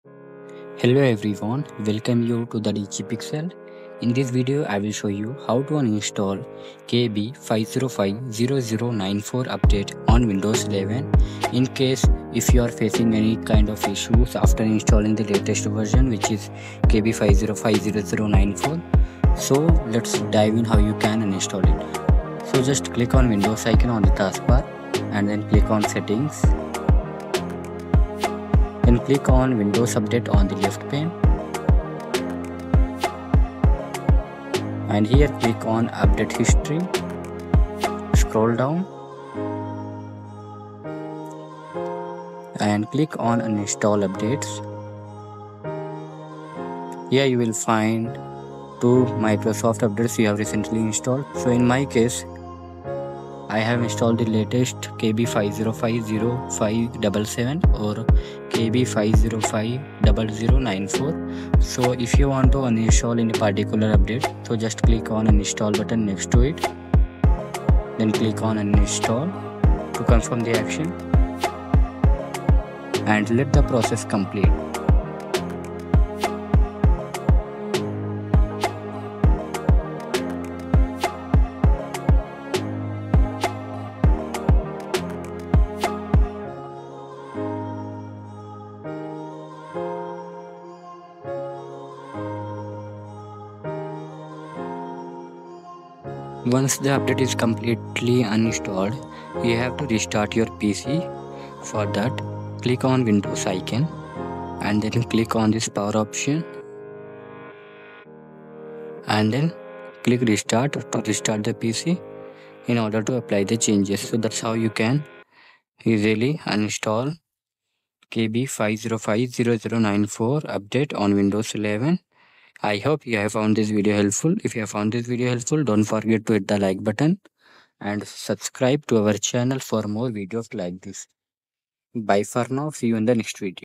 How to Uninstall KB5050094 Update on Windows 11 (Version 24H2)
Are you facing issues after installing the latest Windows 11 24H2 update which is KB5050094/KB5050577 on Windows 11, the only option left is to uninstall or remove the recently installed update from your Windows PC. Follow the steps discussed in this tutorial to uninstall Windows update.

F O R  B U I S N E S S  E N Q U I R I E S